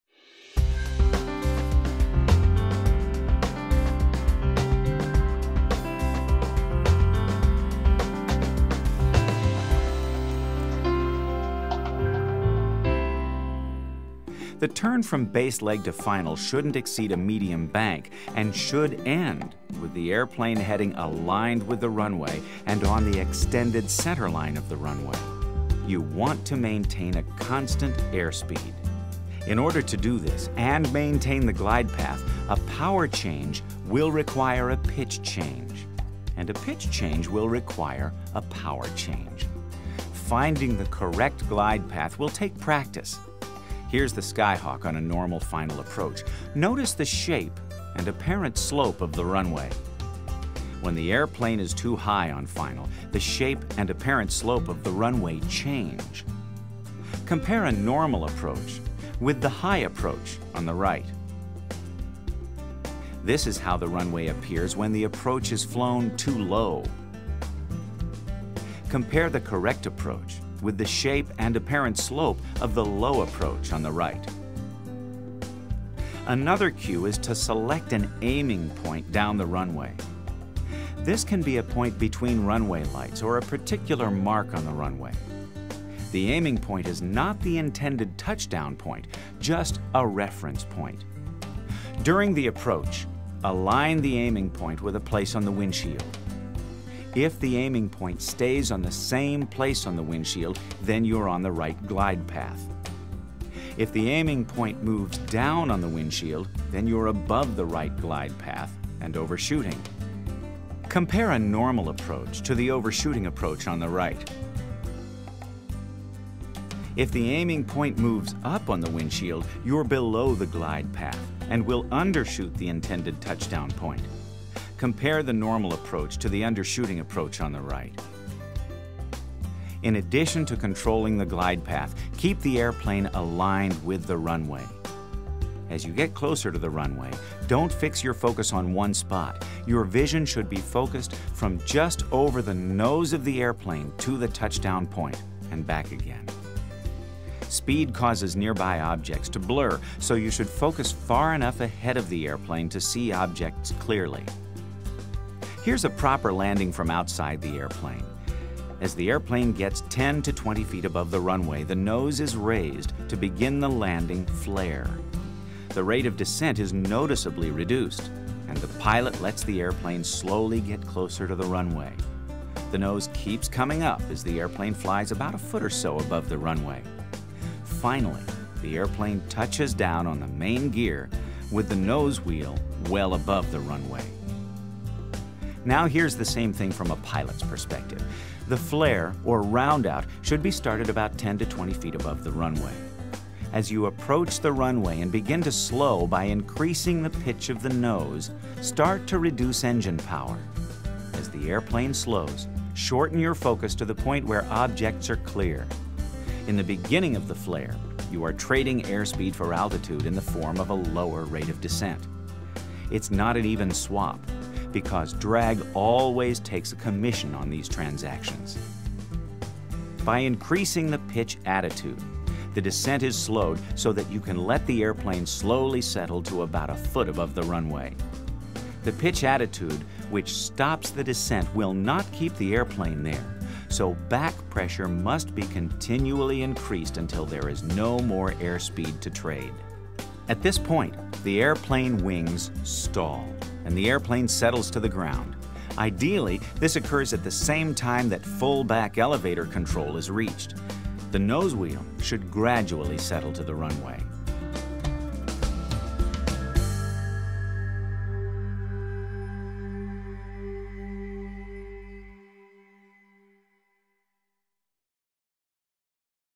Landing an Airplane
One of the most challenging, but rewarding tasks in the flight training process is learning to land. This video shows the basic procedure for landing a Cessna 172 Skyhawk, with exciting footage shot from all around and inside the aircraft during a normal landing.

This is a small segment from Sporty's Pilot Shop's 14 Hour Complete Flight Training Video Course.